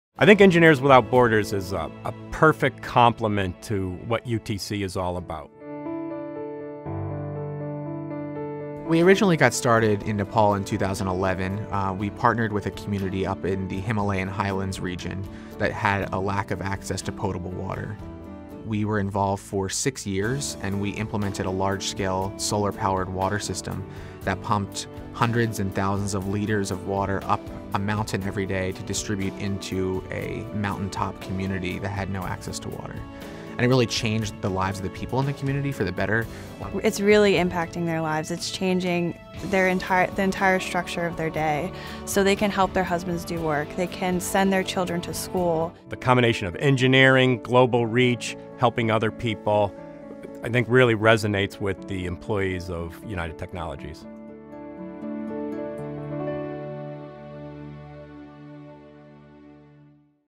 Pratt & Whitney engineers help restore communities worldwide
Learn more about how Pratt & Whitney engineers play an active role in Engineers Without Borders, helping restore communities around the world through key projects like building a large-scale solar-powered water system for the people of Nepal.